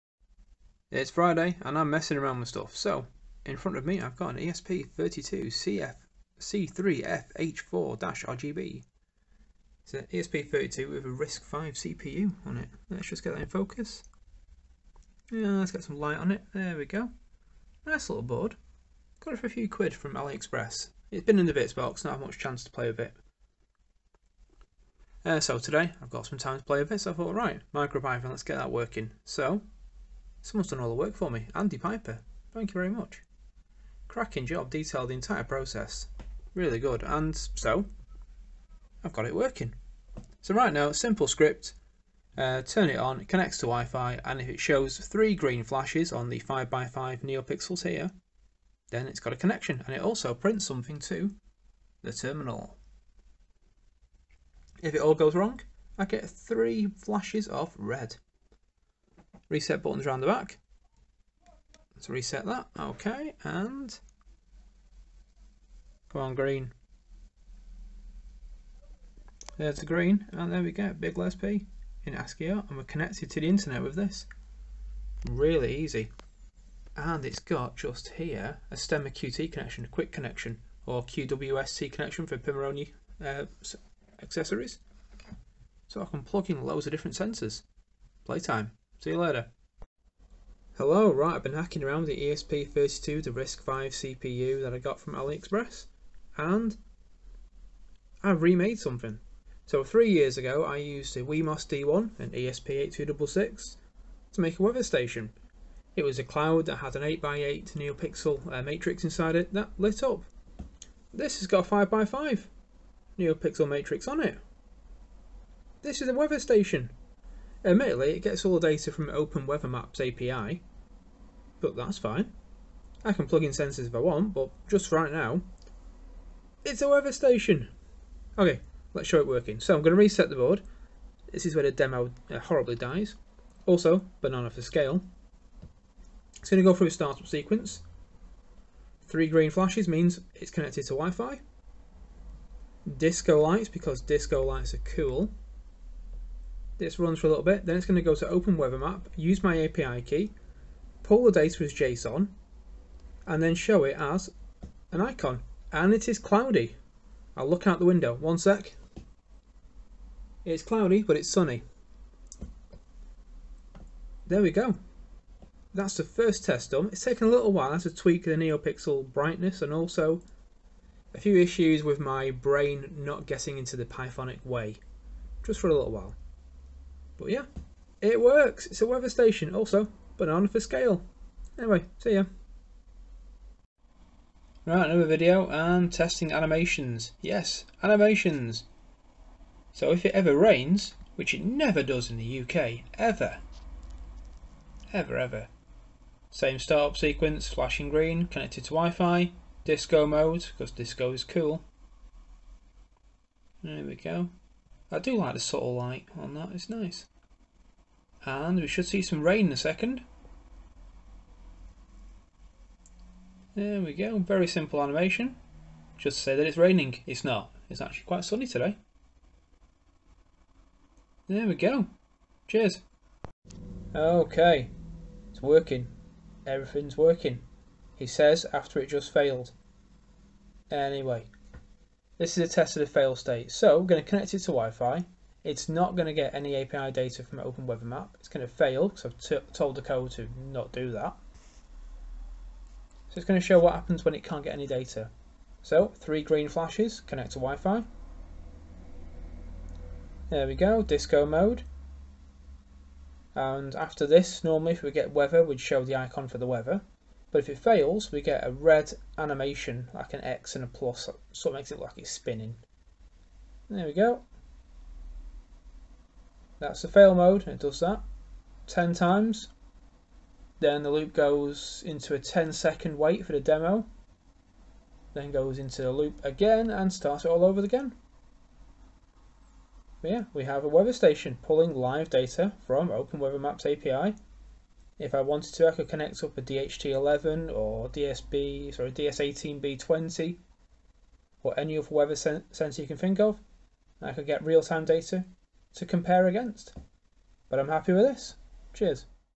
Hacking around with the ESP32-C3FH4-RGB RISC-V Board
These are a series of videos that I released on YouTube, documenting the process of learning the ESP32-C3 board as I built a simple weather station.